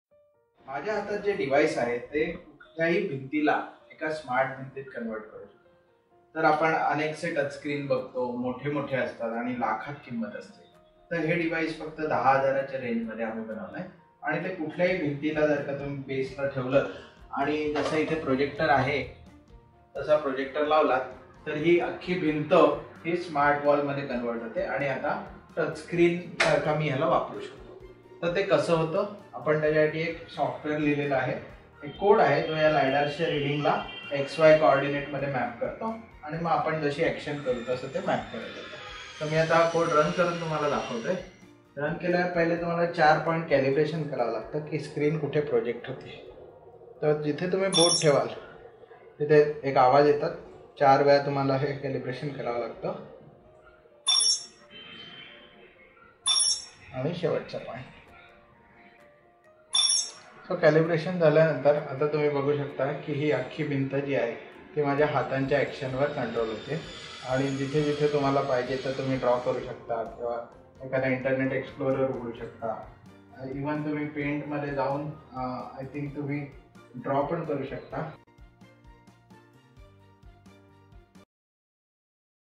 Lidar Smart-wall explained in Marathi
Dr. Ninad and his team created a Lidar-based smart wall. Special Thanks to team members Rutwik, Arnav, Shreyas, Srushti, Ruchi and others.